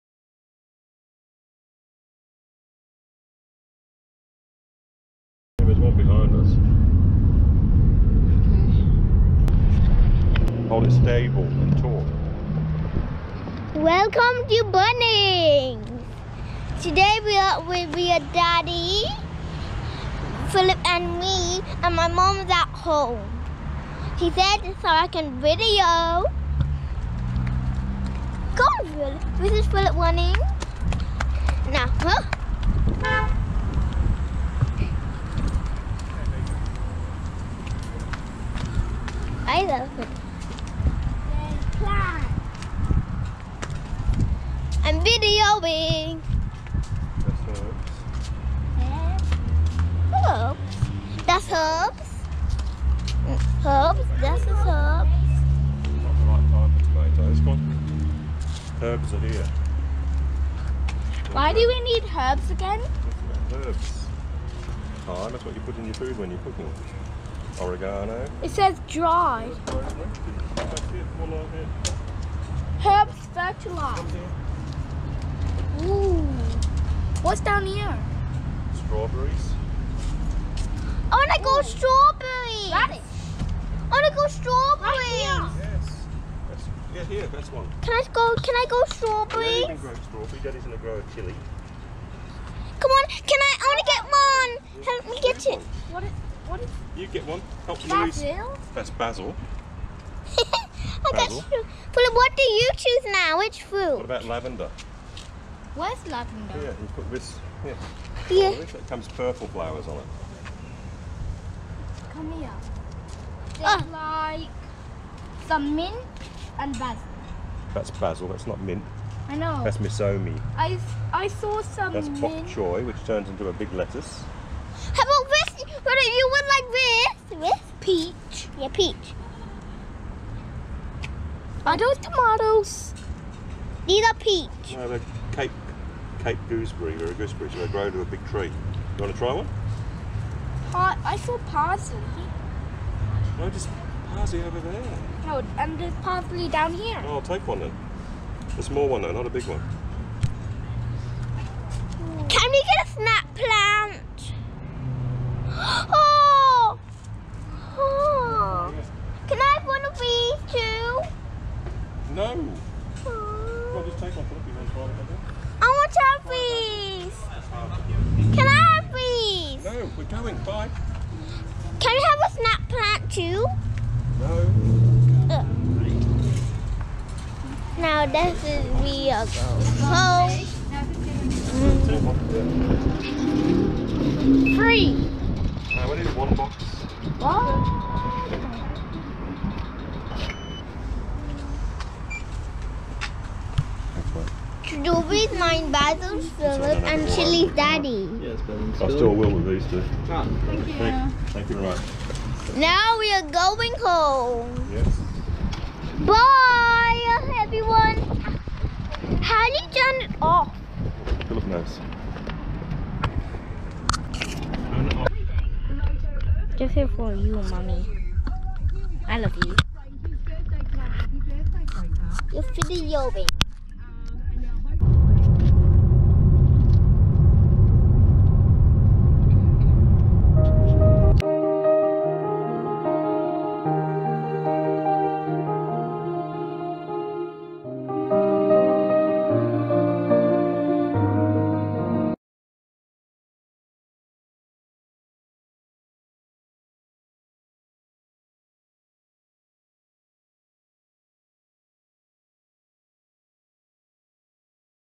Lulu's Vlog Bunnings Warehouse | Yang Pink Minta Dibeli Semua
Memaksa mau ngevlog sendiri di dalam toko saat mampir sejenak, keranjngan melihat warna yang serba pink di toko bunga dan bangunan serba ada.

Forcing herself to vlog in the shop when stopping by for a moment, she's excited to see the all-pink colors in flower shops and convenience buildings.